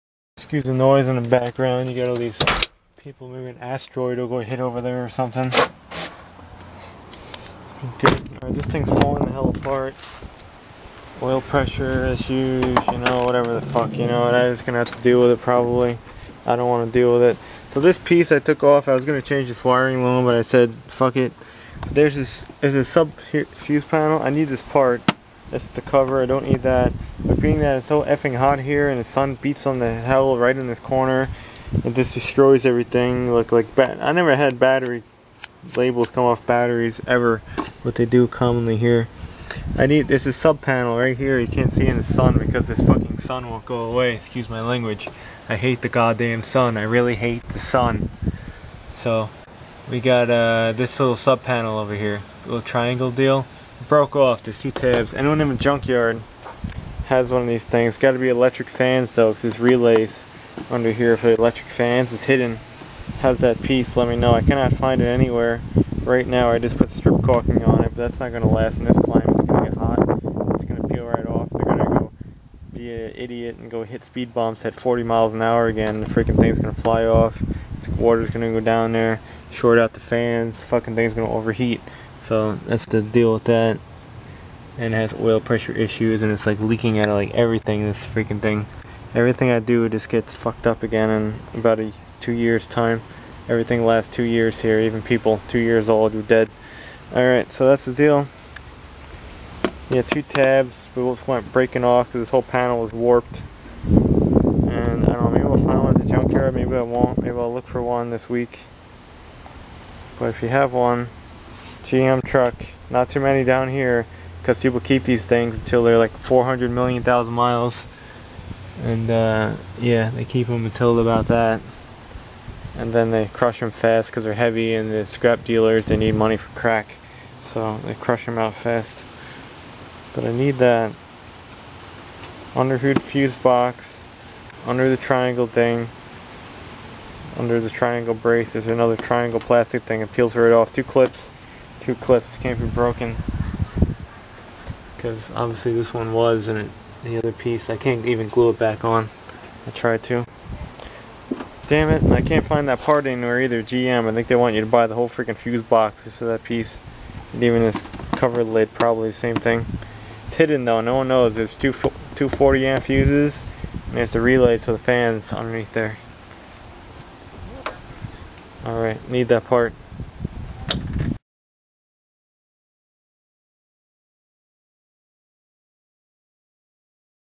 Need that part Stupid truck GMC Chevy Fuse cover
Maybe ill find it but I prefer one from a super cold climate because it won't be all F'd up from the hot heat like it here.